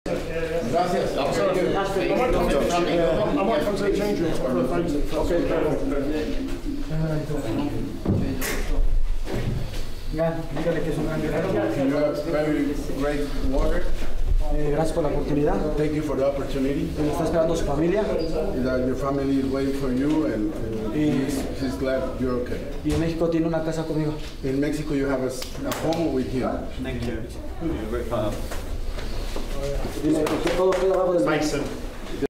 Leigh Wood & Mauricio Lara embrace in medical room post-fight 🤝
Watch as Leigh Wood and Mauricio Lara cross paths in the medics room after their WBA World Featherweight Title in Nottingham on Saturday 18th February 2023...